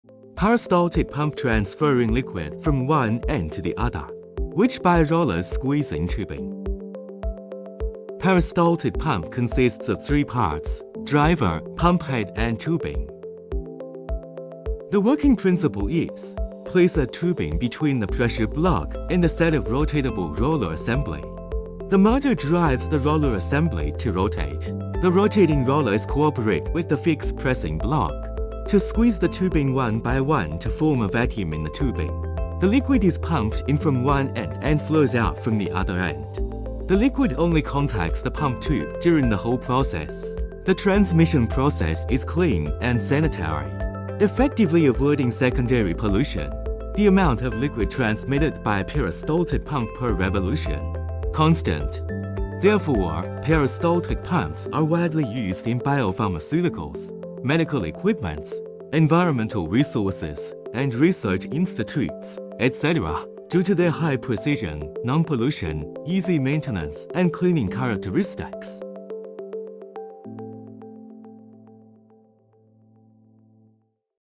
How do peristaltic pumps work? Shenchen Pump
The rotating rollers cooperate with the fixed pressing block to squeez the tubing one by one to form a vacuum in the tubing, the liquid is pumped in from one end and flows out from the other end. The liquid only contacts the pump tube during the whole process. The transmission process is clean and sanitary, effectively avoiding secondary pollution.